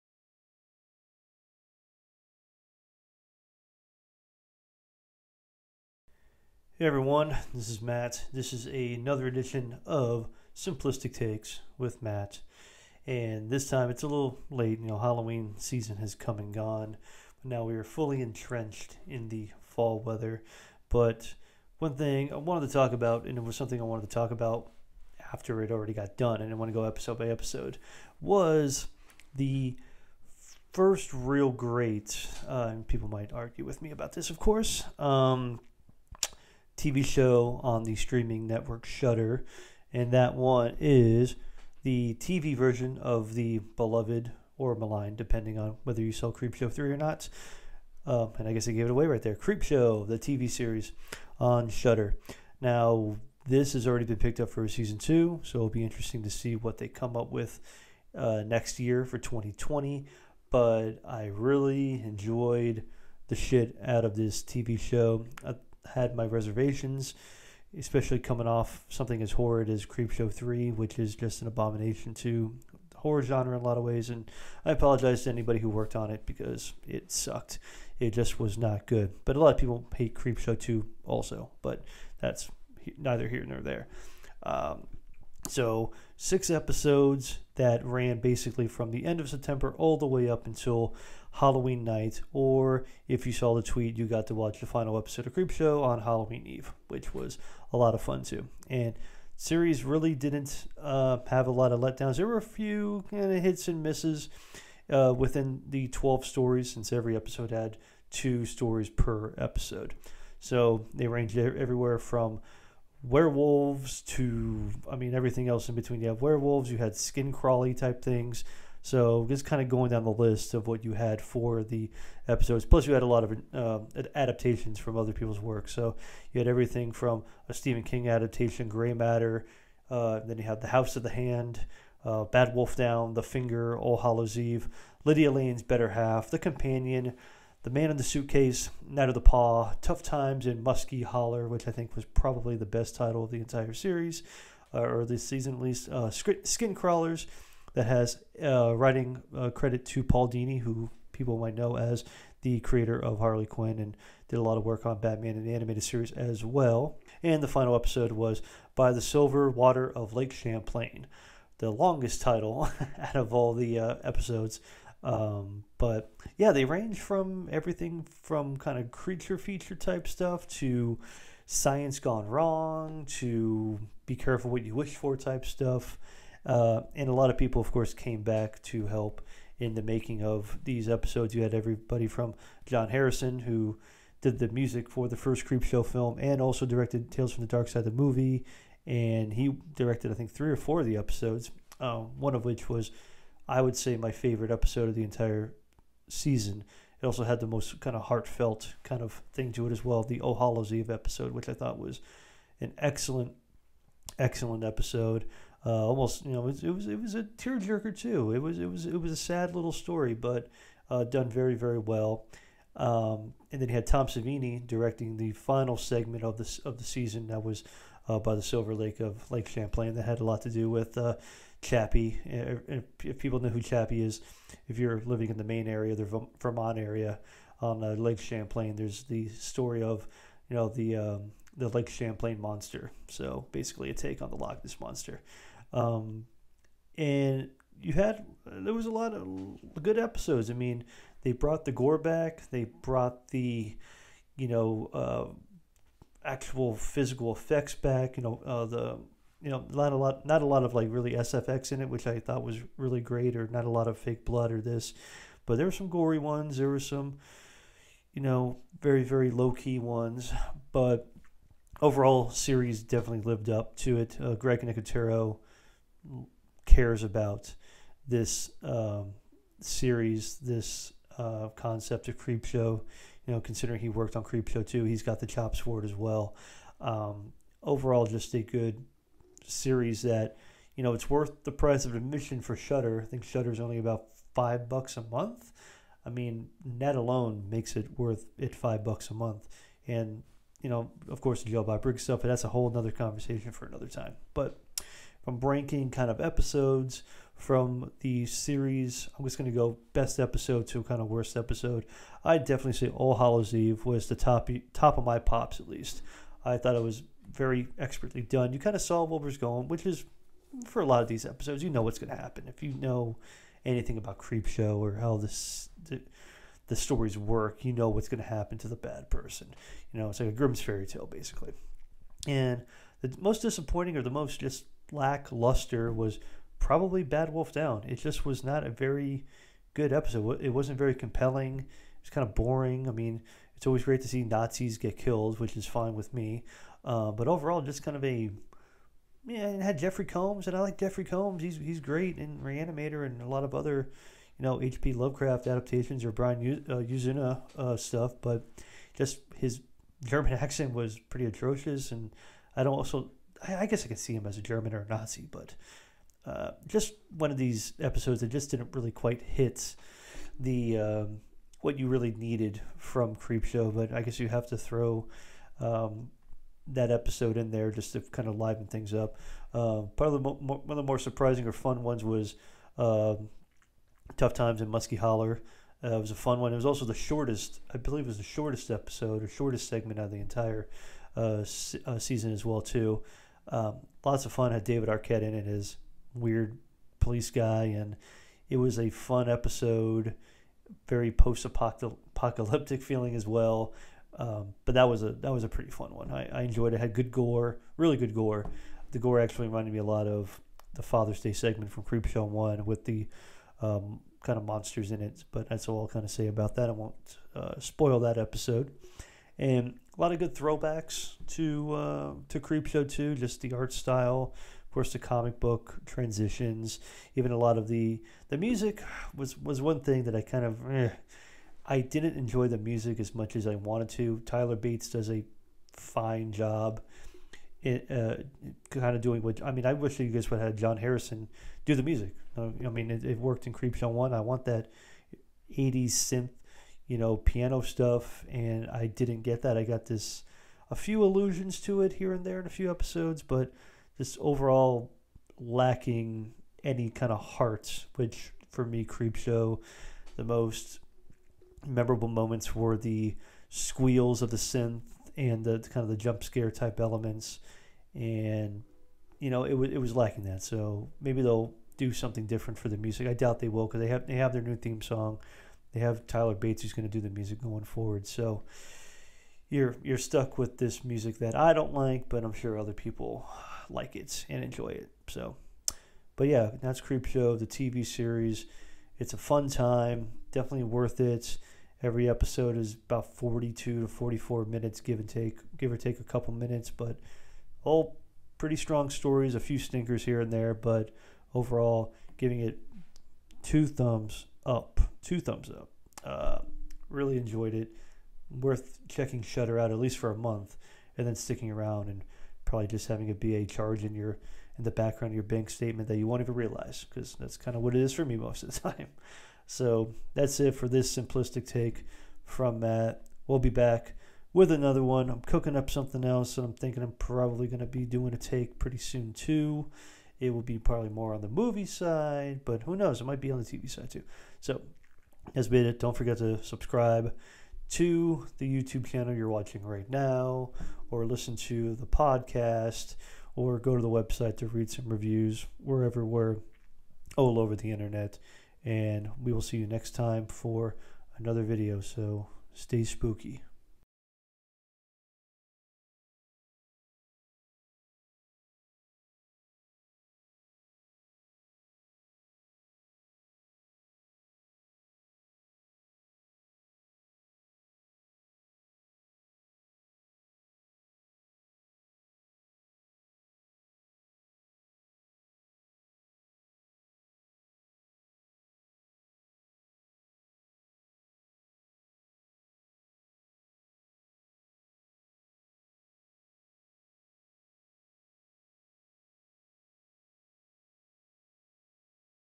Simplistic Reviews Presents: Simplistic Takes - Ep. 3 Creepshow, Season One
Halloween has come and gone, but there's no wrong time to enjoy "Creepshow" over on Shudder!

With 12 creepy stories in Season One, you have werewolves, aliens, diet-loving leeches and plenty more!